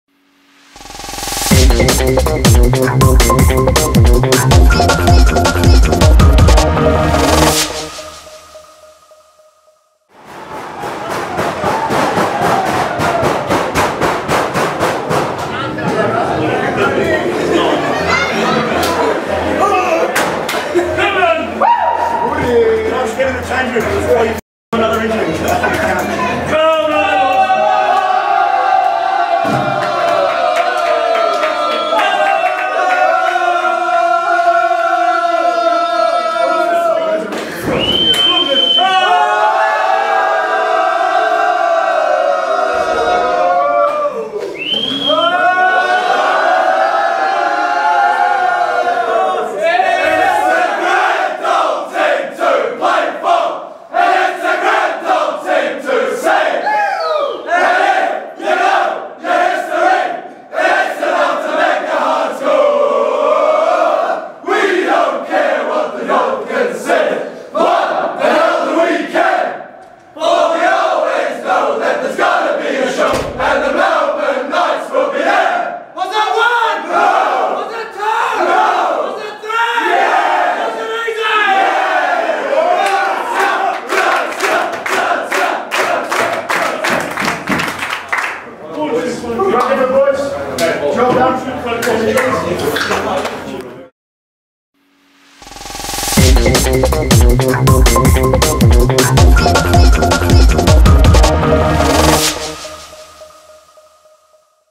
BEHIND THE SCENES: Post-match MKvNC celebrations
The KNIGHTStv cameras were allowed inside the Knights Stadium change rooms after Andrew Marth's men took all three points from Northcote City in the Club's first NPL Victoria match.